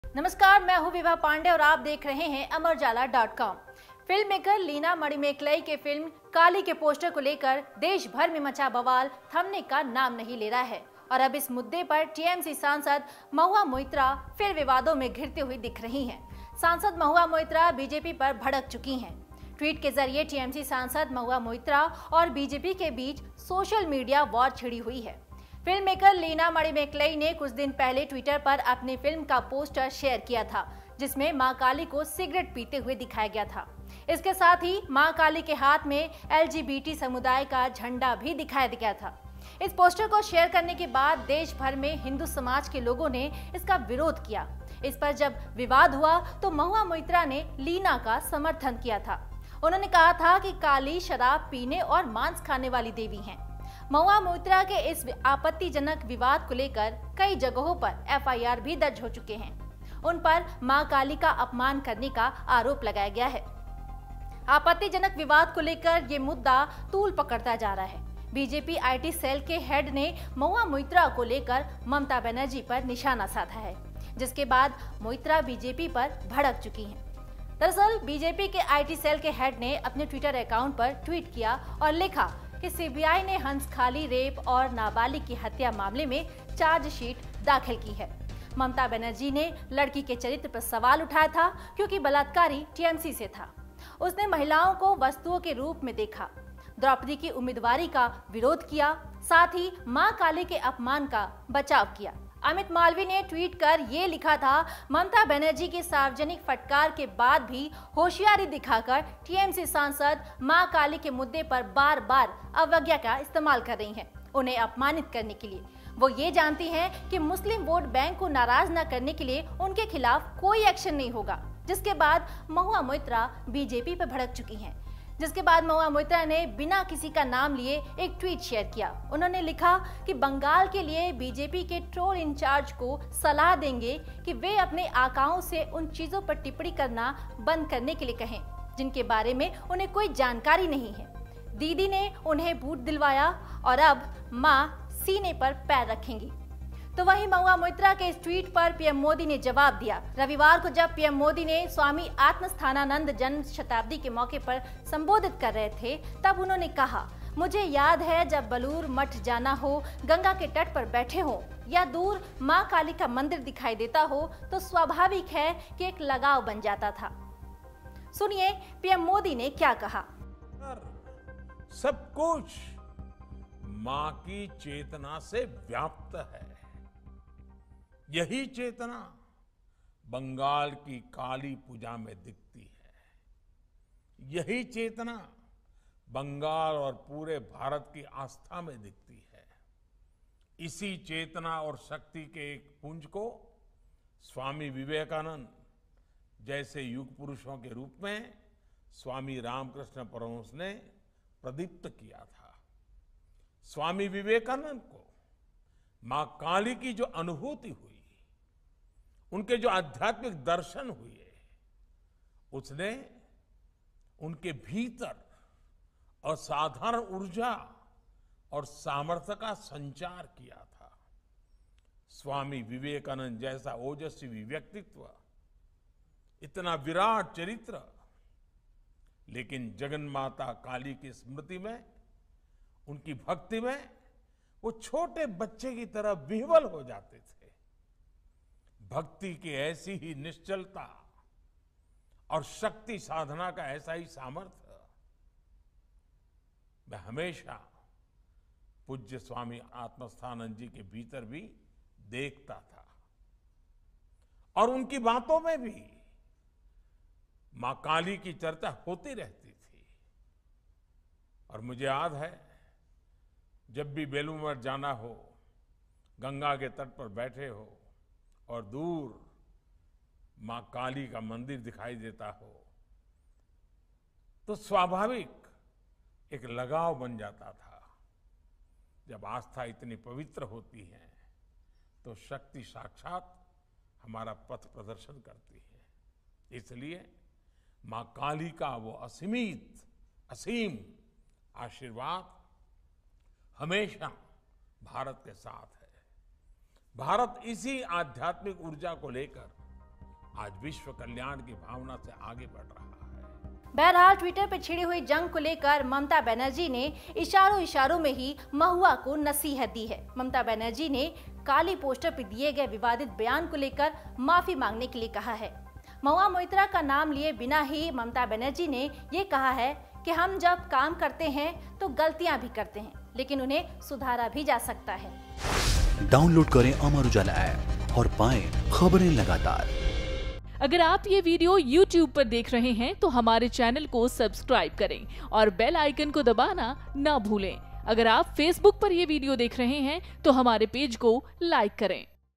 Kaali Poster Controversy: BJP पर भड़कीं Mahua Moitra, ट्विटर पर छिड़ी वॉर | Leena Manimekalai
Kaali Poster Row: फिल्ममेकर लीना मणिमेकलई के फिल्म 'काली' के पोस्टर को लेकर देश भर में मचा हुआ बवाल थमने का नाम नहीं ले रहा है...और अब इस मुद्दे पर tmc सांसद महुआ मोइत्रा फिर विवादों में घिरती हुई दिख रही हैं..सांसद महुआ मोइत्रा बीजेपी पर भड़क चुकी हैं...ट्वीट के जरिए TMC सांसद महुआ मोइत्रा और बीजेपी के बीच सोशल मीडिया वॉर छिड़ी हुई है.
फिल्ममेकर लीना मणिमेकलाई ने कुछ दिन पहले ट्विटर पर अपनी फिल्म का पोस्टर शेयर किया था...जिसमें मां काली को सिगरेट पीते हुए दिखाया गया था, इसके साथ ही मां काली के हाथ में LGBT समुदाय का झंडा भी दिखाया गया था

The nationwide ruckus over the poster of filmmaker Leena Manimekalai's film 'Kali' is not taking its name to stop and now TMC MP Mahua Moitra is again embroiled in controversies on this issue. MP Mahua Moitra is furious at BJP. Through the tweet, a social media war has broken out between TMC MP Mahua Moitra and BJP.

Filmmaker Leena Manimekalai shared the poster of her film on Twitter a few days ago, in which Maa Kali was shown smoking a cigarette, along with the flag of the LGBT community in Maa Kali's hand.